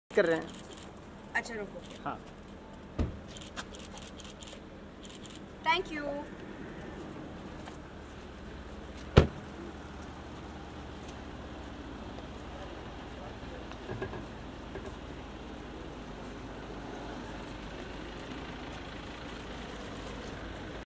Parineeti Chopra Spotted At Priyanka Chopra Hobollywood filmuse In Juhu
Watch Parineeti Chopra Spotted At Priyanka Chopra House In Juhu